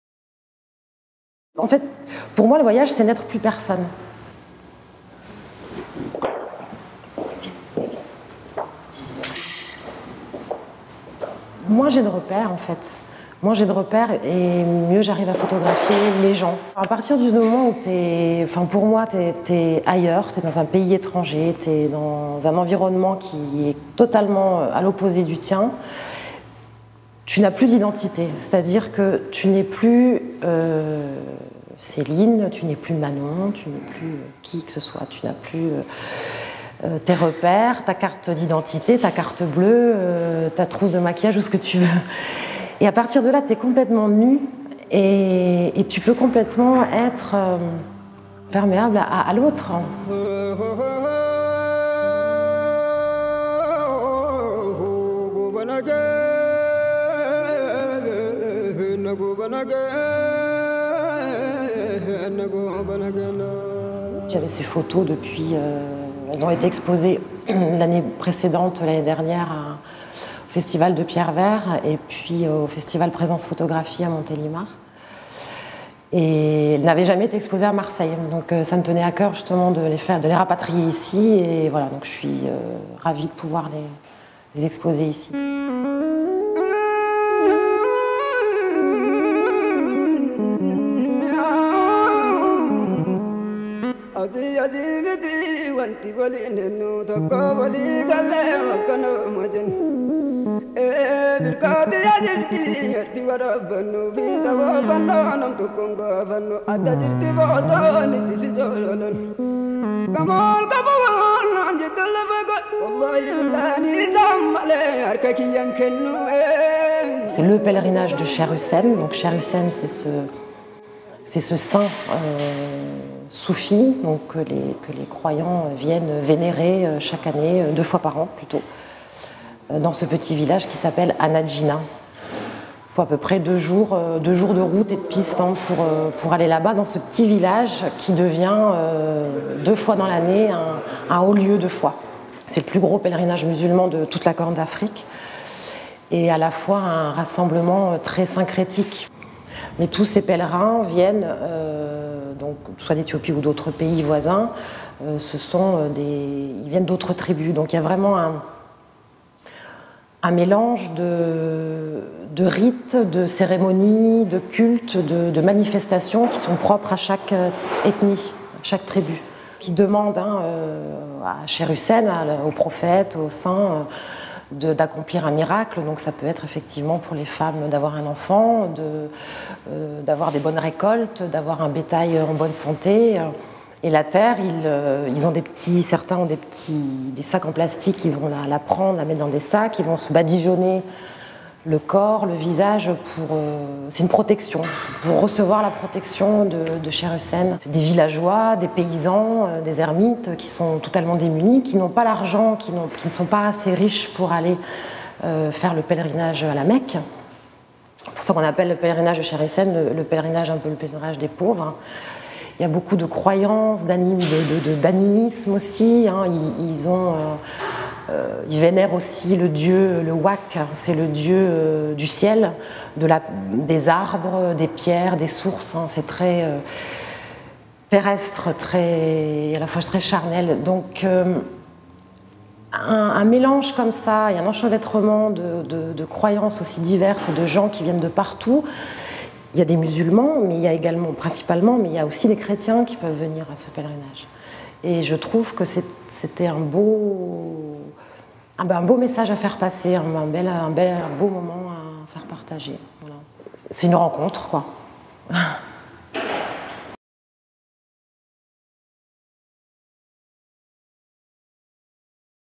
Céline Ravier : Ferveur, les mystiques d'Annajina.
Éthiopie. Dans la province montagneuse du Balé, se tient depuis des siècles le plus important pèlerinage musulman de toute la corne d'Afrique. Venant d’Éthiopie, du Kenya, de Somalie ou d’Érythrée, des dizaines de milliers de pèlerins, trop pauvres pour effectuer le pèlerinage vers la Mecque, convergent vers le minuscule mausolée de Cheikh Hussein, grande figure de l’introduction de l’islam dans les immenses territoires Oromos. Autour du tombeau de ce saint, vénéré pour ses miracles, à la croisée de l'islam et des croyances africaines, le petit village perdu d’Annajina devient alors deux fois l’an un haut lieu de foi, rassemblant manifestations et rites propres à chaque appartenance tribale.